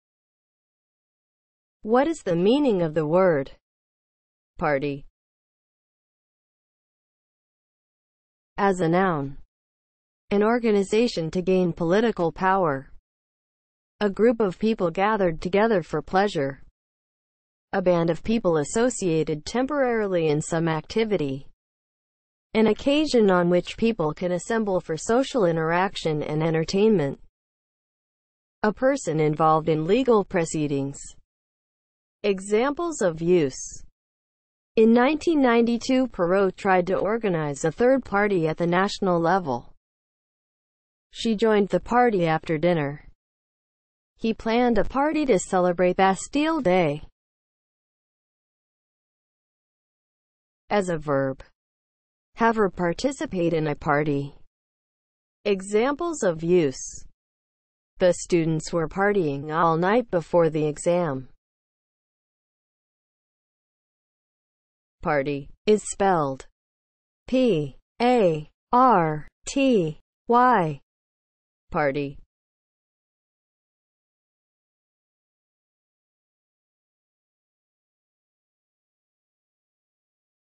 What is the meaning of the word PARTY?
Definition, examples of use and spelling of the word PARTY.

(00:00) Intro
(00:07) As a noun
(00:49) As a verb
(01:03) Spelling

As a noun (00:07):
• an organization to gain political power
• a group of people gathered together for pleasure
• a band of people associated temporarily in some activity
• an occasion on which people can assemble for social interaction and entertainment
• a person involved in legal proceedings

Examples:
• "in 1992 Perot tried to organize a third party at the national level"
• "she joined the party after dinner"
• "he planned a party to celebrate Bastille Day"

As a verb (00:49):
• have or participate in a party

Examples:
• "The students were partying all night before the exam"